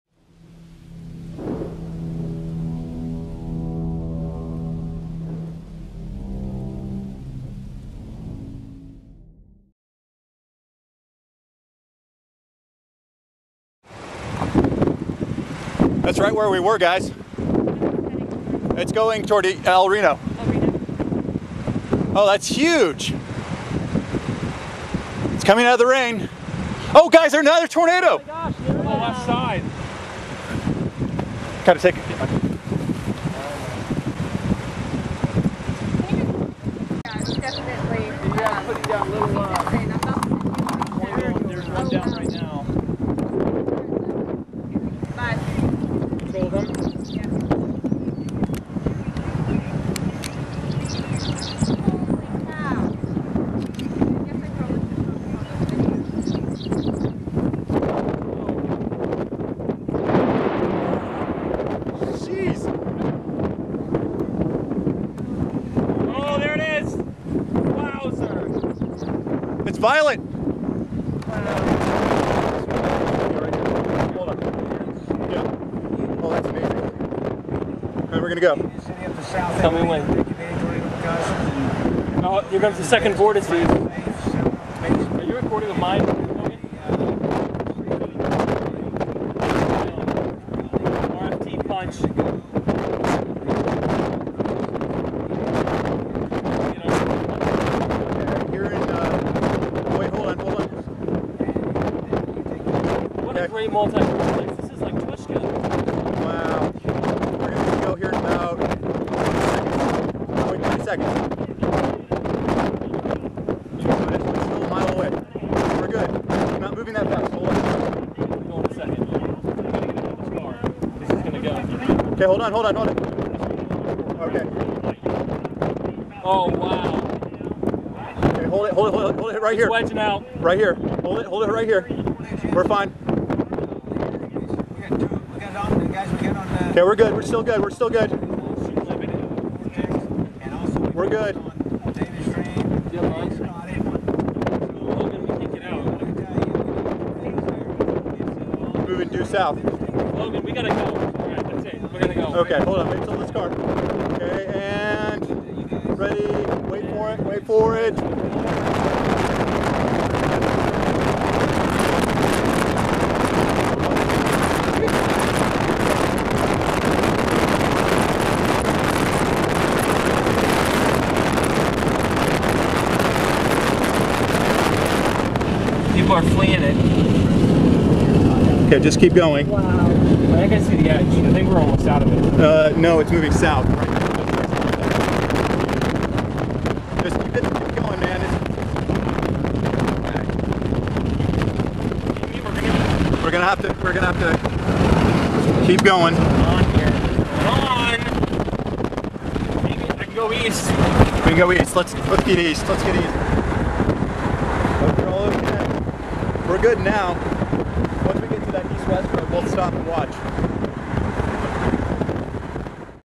Evolution of the world-record EF-5 El Reno, Oklahoma tornado on May 31, 2013
Storm chasers Gabe Garfield, Tim Marquis, Amy Edmonds and Logan Karsten documented the evolution of a massive multi-vortex-to-wedge tornado on May 31, 2013.  The tornado and its very dangerous parent thunderstorm complex was responsible for considerable damage to life and property in the Oklahoma City metro and surrounding communities.  The extremely large wedge tornado maxed out a size of at least 2.6 miles, making it the largest ever recorded in North America; it was also given a rating of EF-5, the highest on the Enhanced Fujita scale.  Sadly, this was also the same tornado that took the lives of our friends and fellow chasers Tim Samaras, Paul Samaras and Carl Young, as well as at least 15 others.